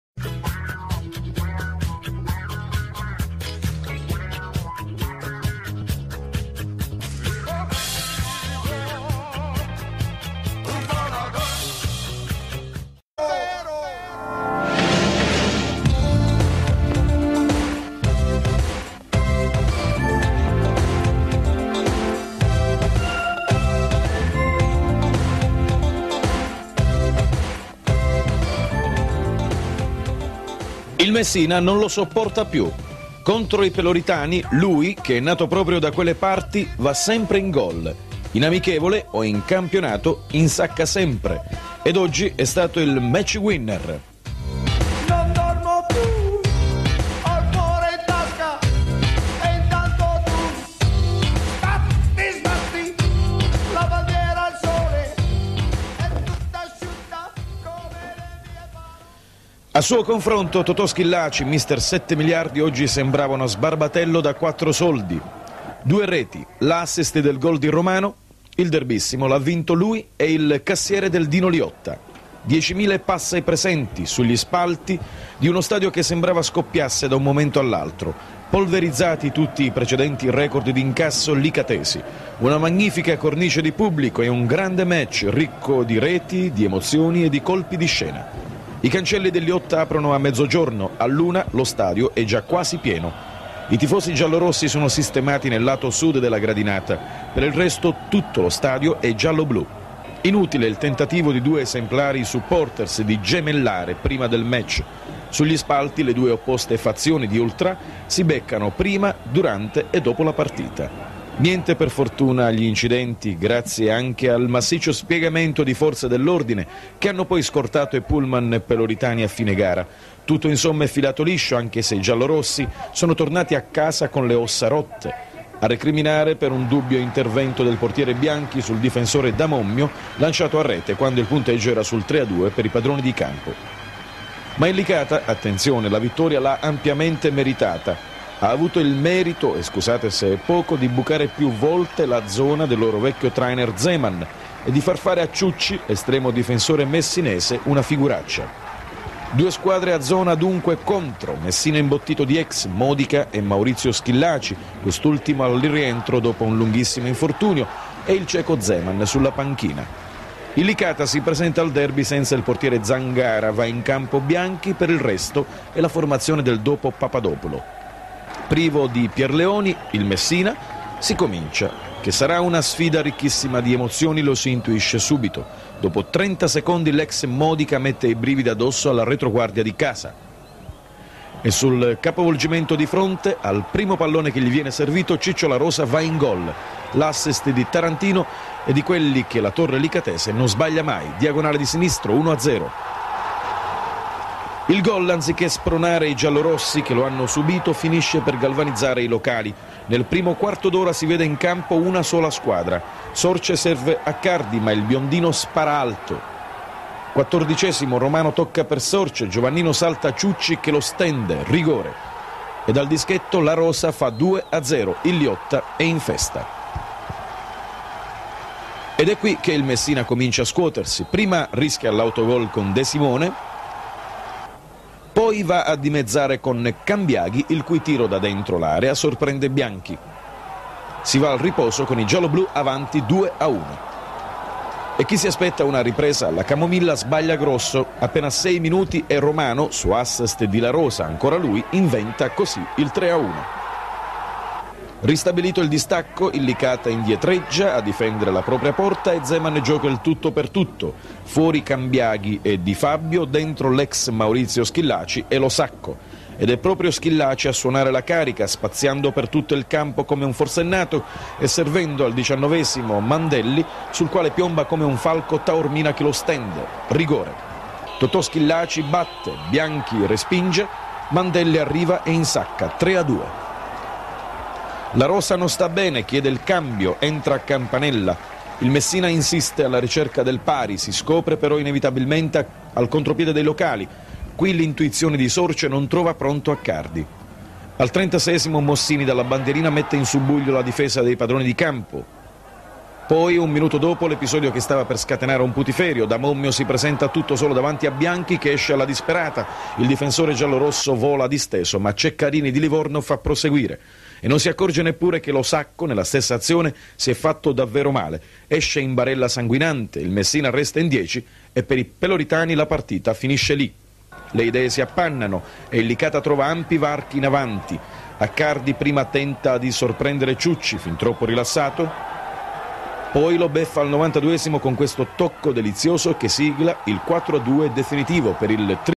1988/89 - Serie B - Licata-Messina 4-2 (2 La Rosa, Romano, Accardi)
Video tratto dalla pagina fb "U Licatisi"